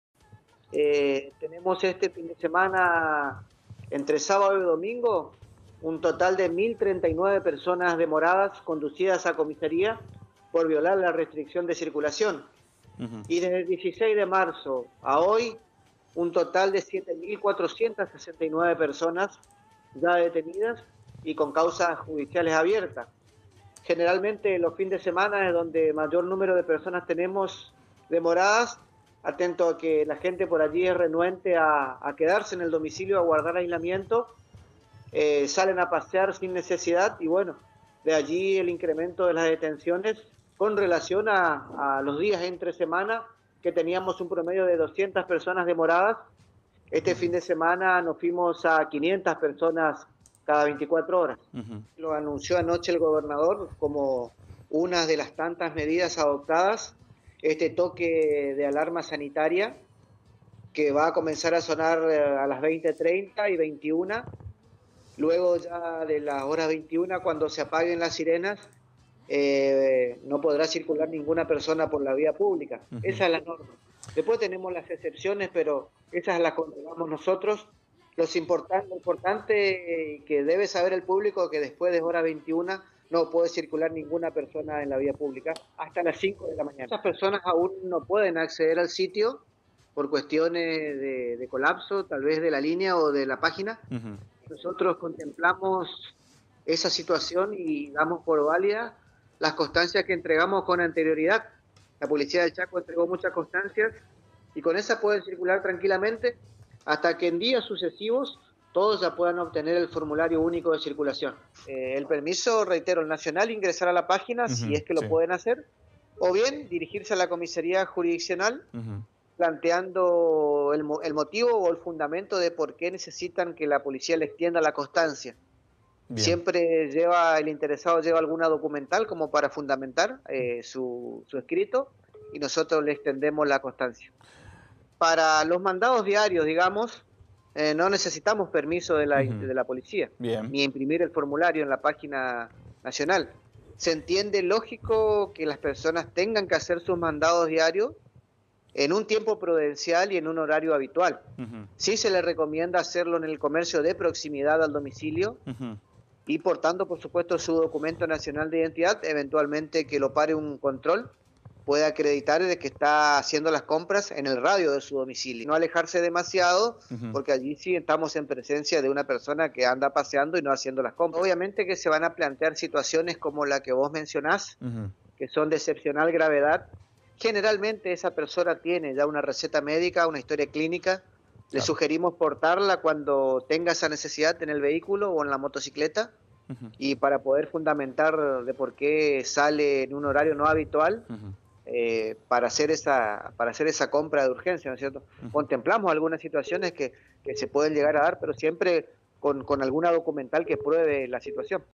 FERNANDO ROMERO - JEFE DE LA POLICIA DEL CHACO
La Policía del Chaco efectuará a las 20:30 horas el 'primer aviso', mientras que a las 21:00 se cumplirá el 'Horario límite'. "Una vez que las sirenas se encuentren encendidas, las personas que no estén autorizadas a circular serán detenidas", se informó el pasado martes cuando se llevó a cabo el simulacro que, desde este lunes, será ejecutado con un amplio operativo por parte de la fuerza de seguridad provincial.